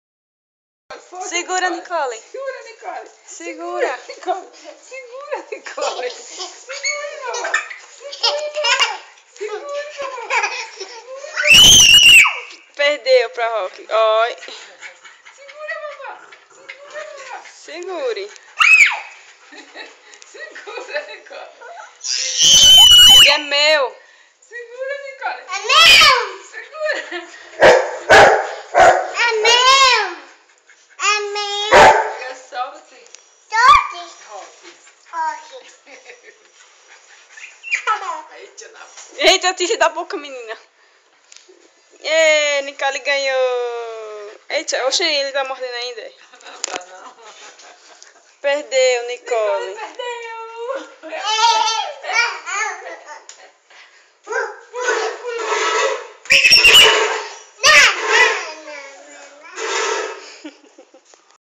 Gatinha x cachorro
Nicolly tentando pegar o urso de estima��o de Rock o cachorro.